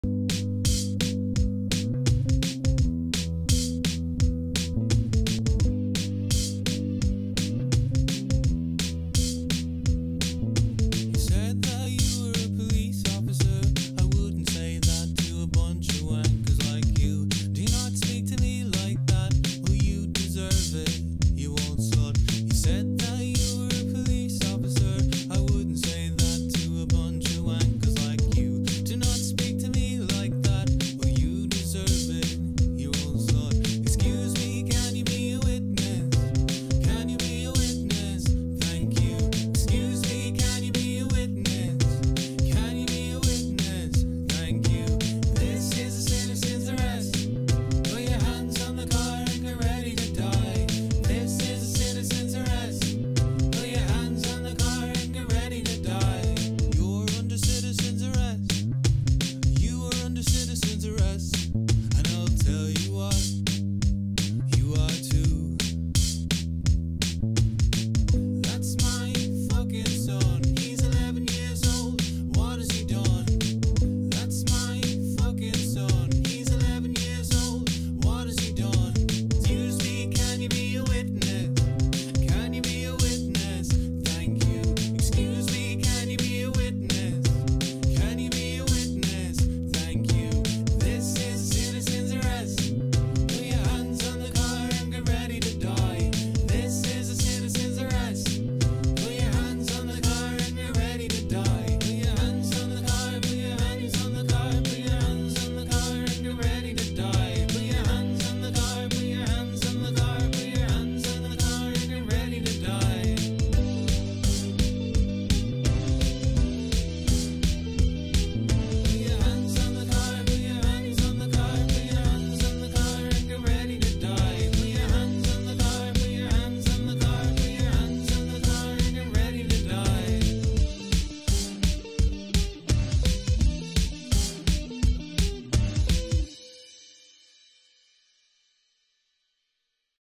This is a Citizens Arrest! But it’s performed by an Indie Band
Put your hands on the car and prepare to die!